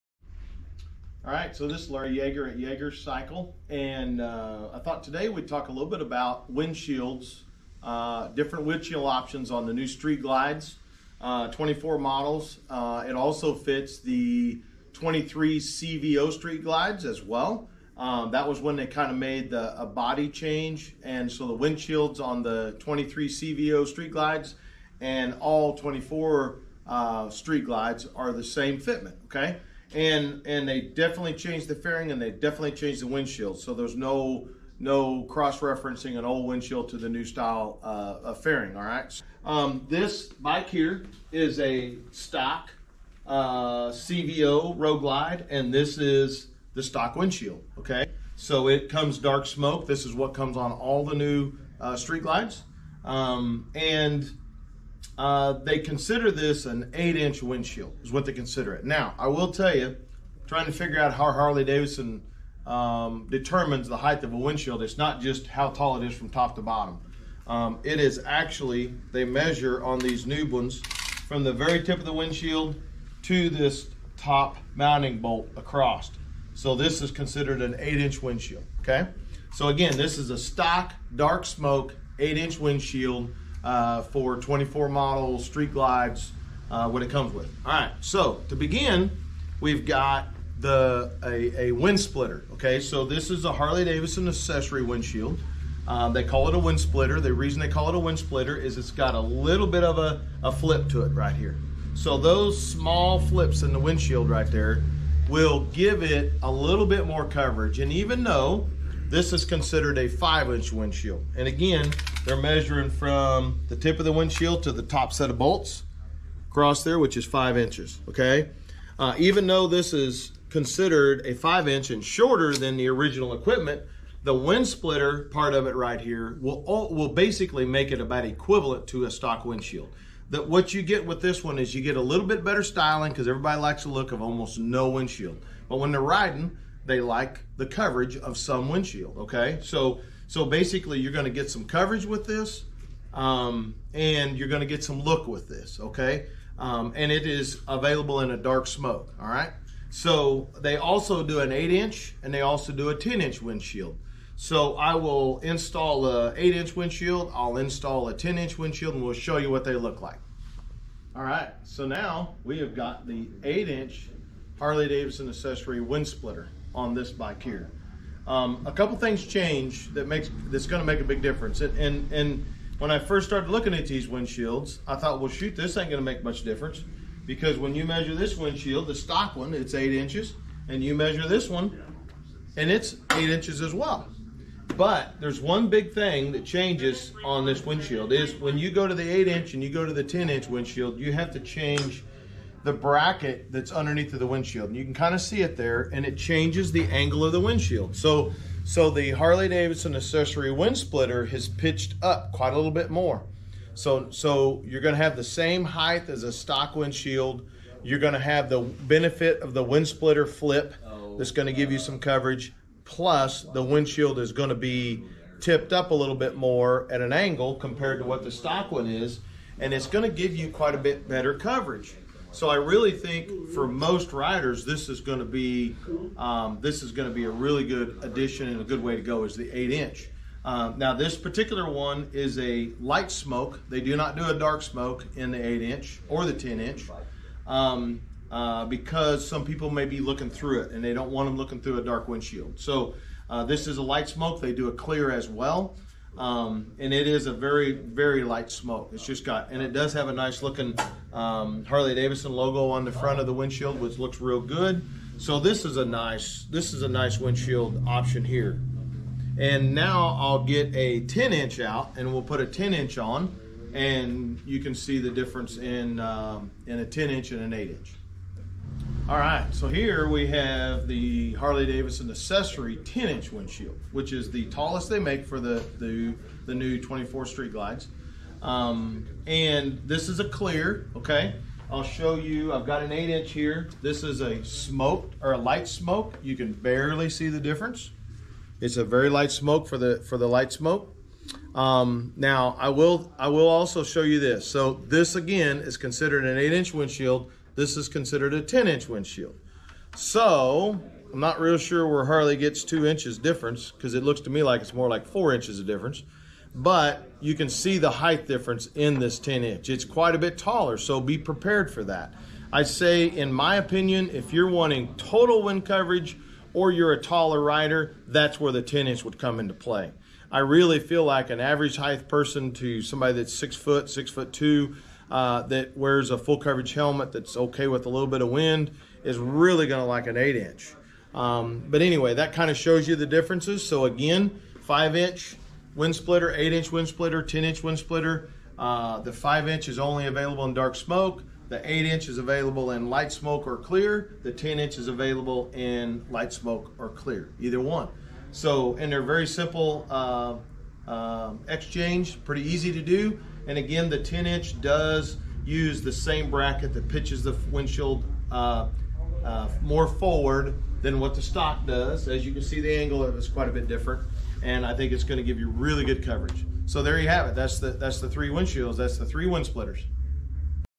Exploring the 3 Windshield Options for the 2024 Harley-Davidson Street Glide!
Welcome to Yeager's Cycle! In this video, we're excited to introduce you to the three different windshield options available for the all-new 2024 Harley-Davidson Street Glide. Whether you're a daily commuter, a weekend warrior, or a long-distance rider, we have the perfect windshield to enhance your riding experience. Watch as we break down the features and benefits of each option.

📍 Visit Us:
Come visit Yeager's Cycle to see these windshields in person and find the perfect one for your 2024 Harley-Davidson Street Glide. Our expert team is here to help you make the best choice for your riding needs.

0:00 introduction
1:00 Stock windshield for 2024 Harley-Davidson Street Glide
1:28 2024 Harley-Davidson Street Glide 5" wind splitter windshield
2:42 2024 Harley-Davidson Street Glide 8" wind splitter windshield
4:30 2024 Harley-Davidson Street Glide 10" wind splitter windshield